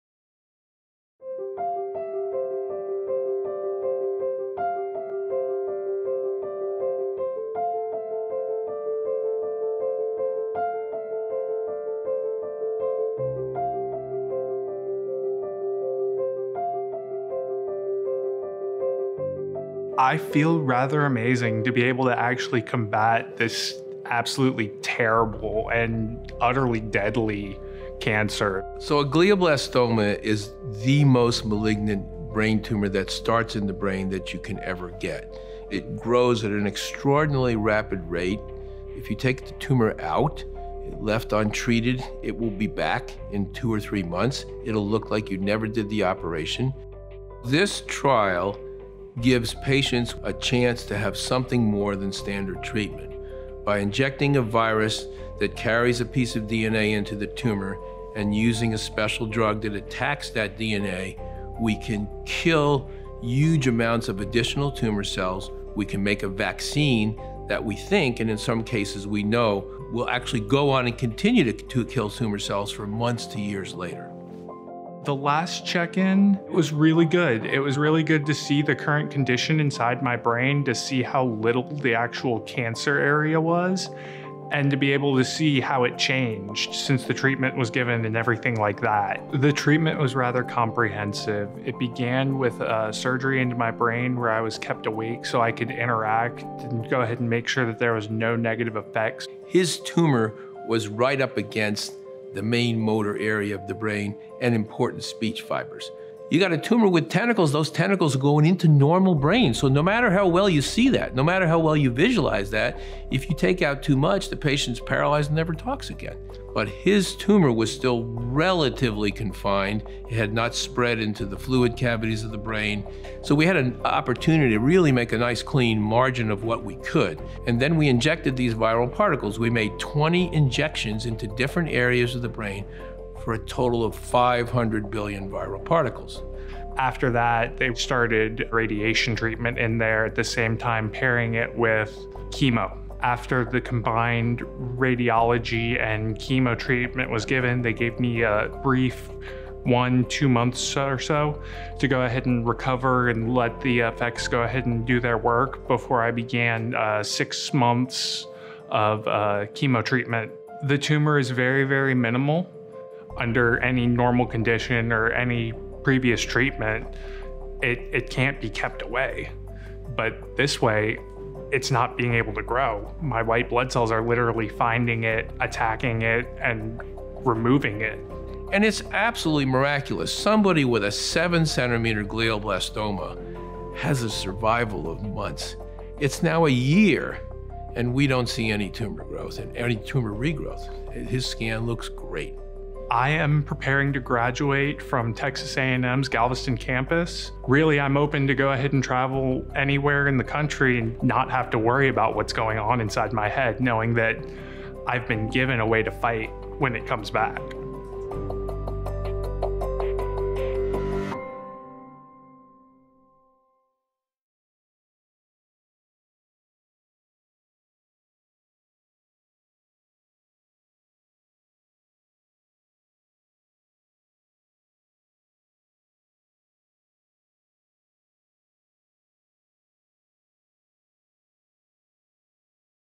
Fighting Glioblastoma with an Experimental Therapy: Stratton’s Story
After being diagnosed with glioblastoma, an aggressive brain tumor, Stratton Muhmel refused to give up. He enrolled in an experimental new treatment that combines brain surgery, gene therapy and radiation. Almost a year and half after his diagnosis, Stratton shares his story.